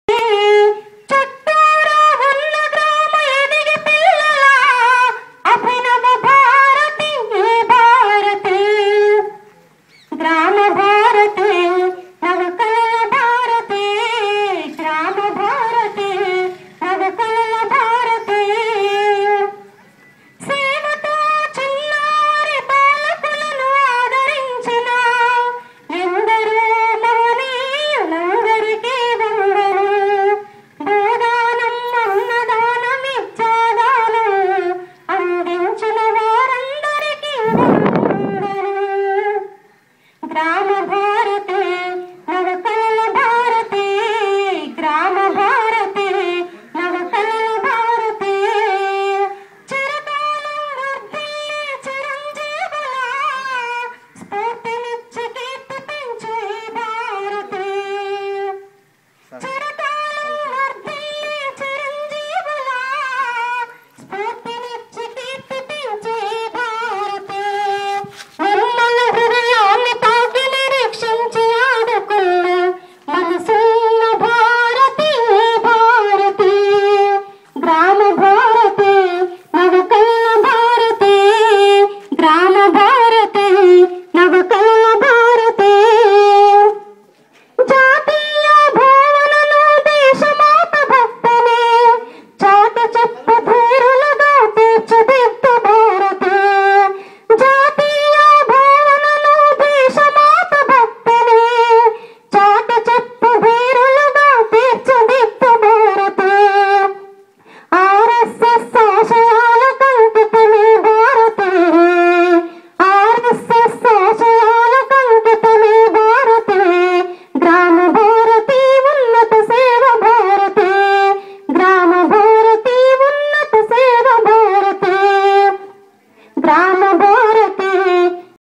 Song on Grama bharathi
Song on Grama bharathi at marrigudem .. Acharya nagarjuna awasam